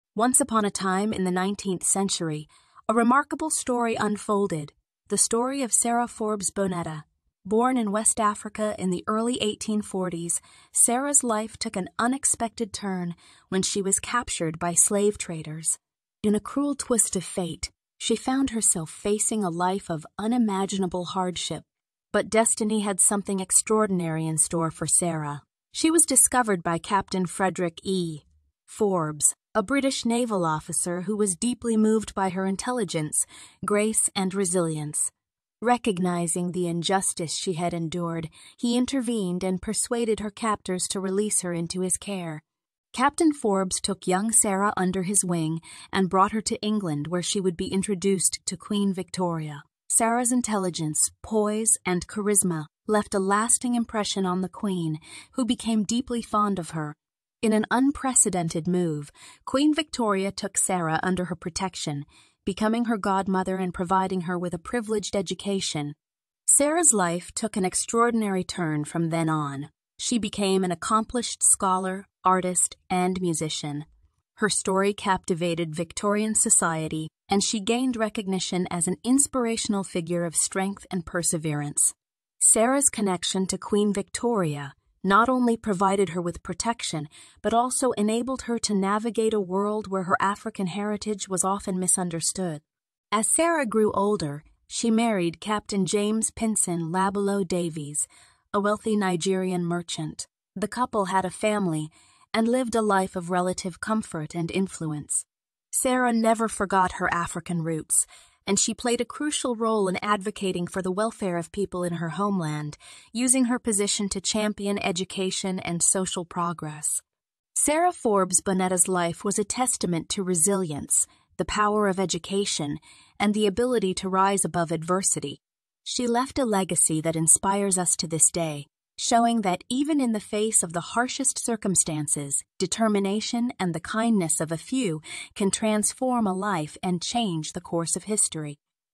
Sarah Forbes Bonetta: From Captive to Royal Protege
Discover the extraordinary journey of Sarah Forbes Bonetta, a West African girl captured by slave traders, who later became a beloved protege of Queen Victoria herself. Witness her triumphant transformation from captive to a remarkable figure in Victorian England, as she defied the odds, embraced her African heritage, and left an inspiring legacy.